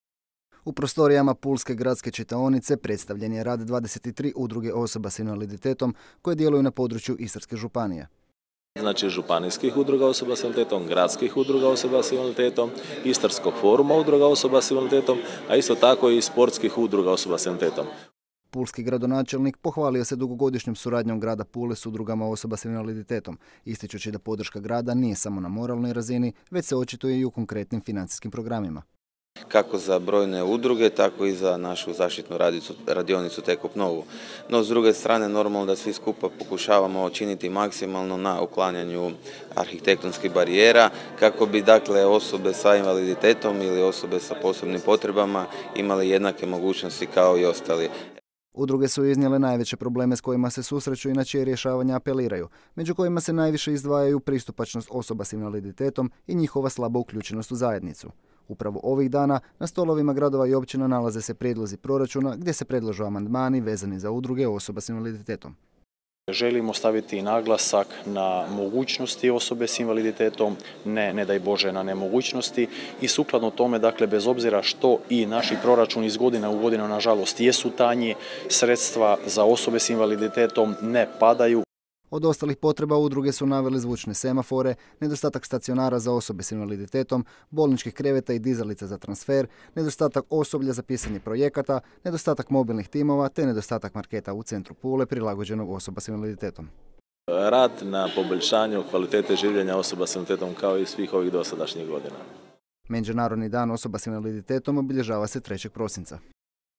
Župan i pulski gradonačelnik primili udruge osoba s invaliditetom Istre
Povodom Međunarodnog dana osoba s invaliditetom danas su pulski gradonačelnik Boris Miletić i istarski župan Valter Flego održali prijem za predstavnike udruga osoba s invaliditetom Grada Pule i Istarske županije. Tom prilikom udruge su lokalnim vlastima predstavile svoje probleme i želje.